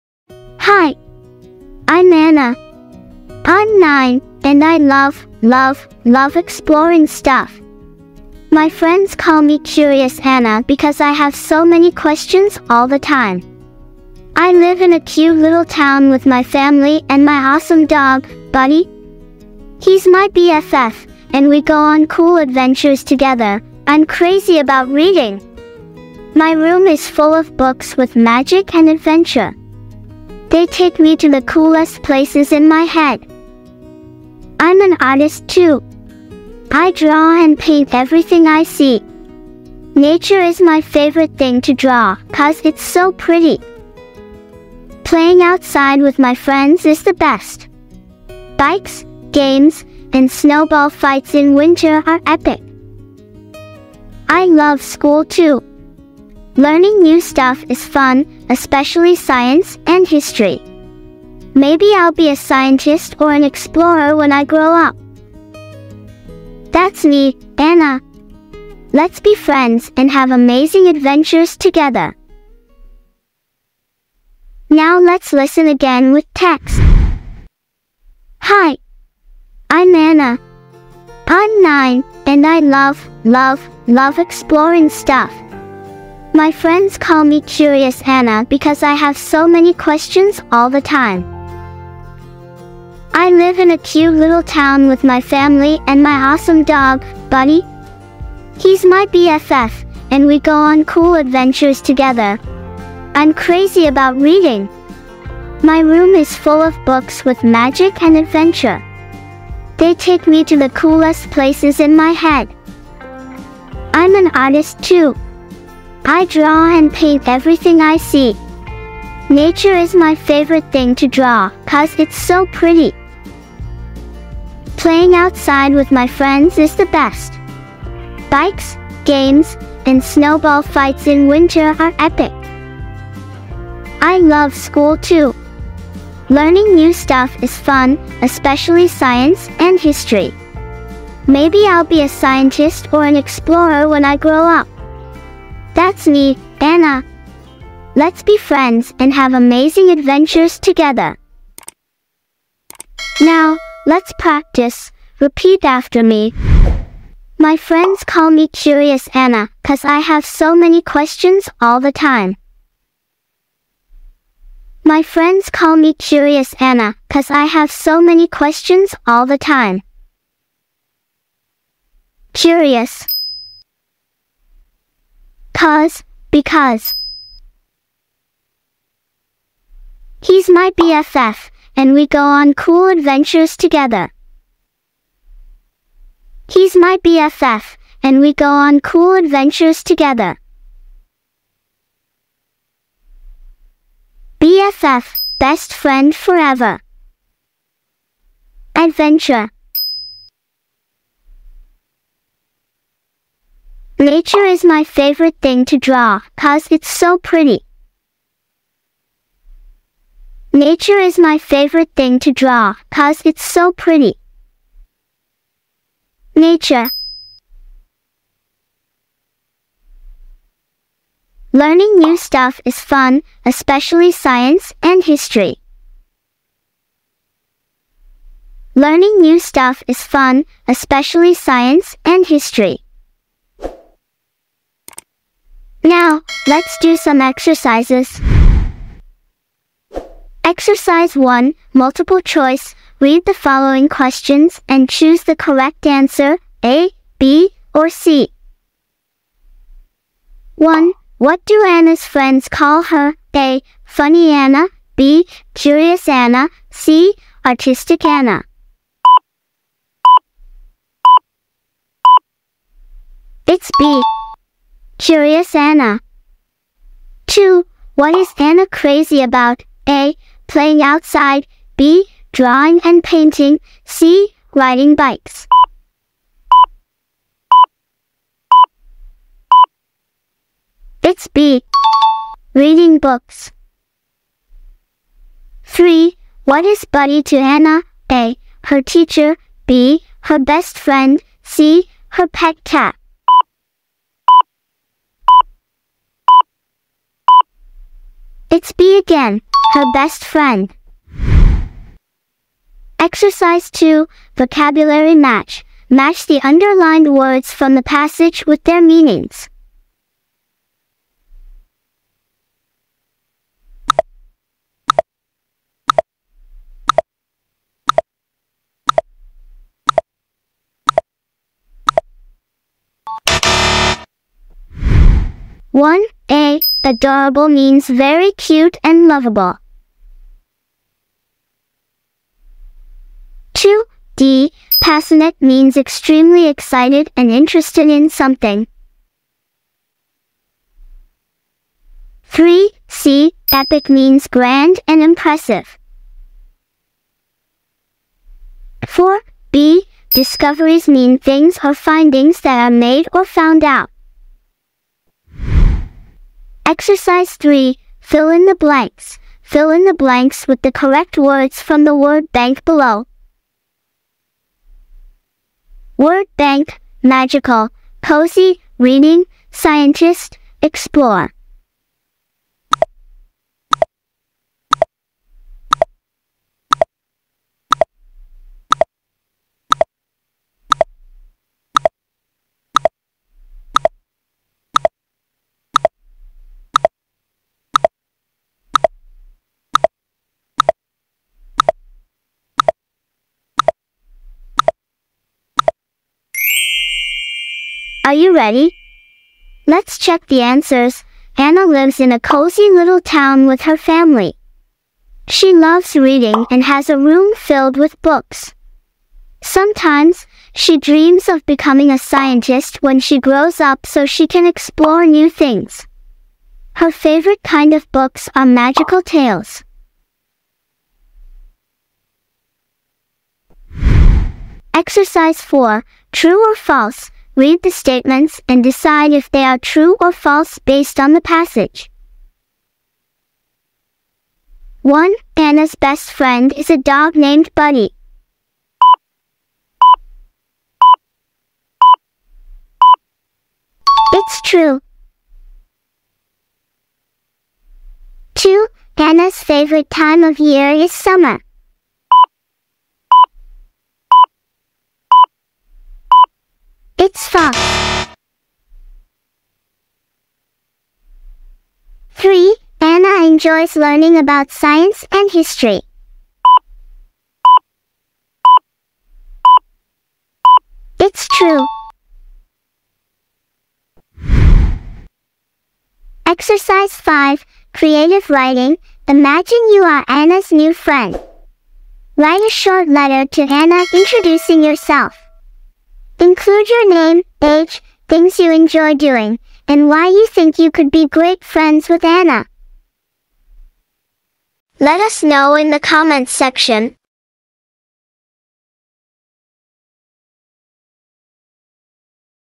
10 minutes English | Curious Anna - a 9-year old girl…
10 minutes English | Curious Anna - a 9-year old girl…

🌟 Meet Anna, the curious 9-year-old explorer! 🌟

Join her on an enchanting adventure filled with books, art, and outdoor fun. Discover the joy of learning English through engaging exercises and exciting friendship journeys. Let's explore together! 🚀📚🎨🌈